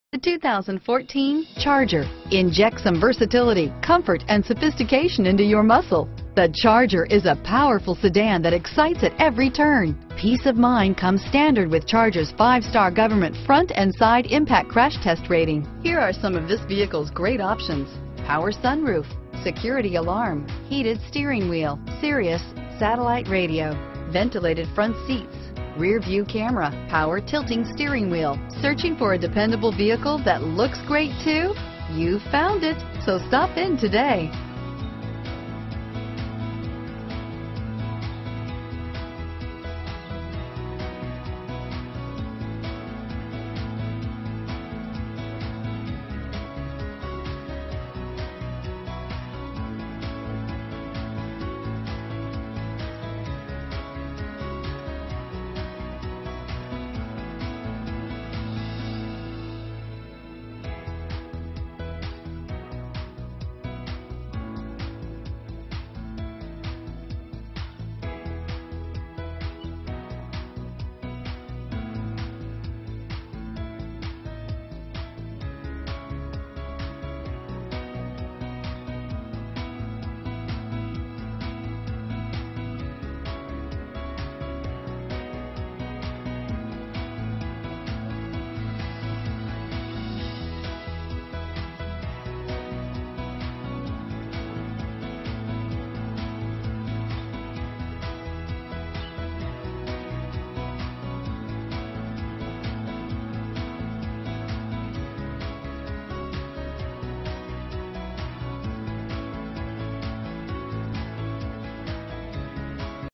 2014 Dodge Charger Boulder, Longmont, Broomfield, Louisville, Denver, CO 14048
2014 Dodge Charger

Valley Dodge
2100 30th St

Step into the 2014 Dodge Charger! A sensational four door destined to dominate on the racetrack and impress on the expressway! Top features include leather upholstery, a built-in garage door transmitter, blind spot sensor, and voice activated navigation. It features an automatic transmission, all-wheel drive, and a powerful 8 cylinder engine. We pride ourselves in the quality that we offer on all of our vehicles. Stop by our dealership or give us a call for more information.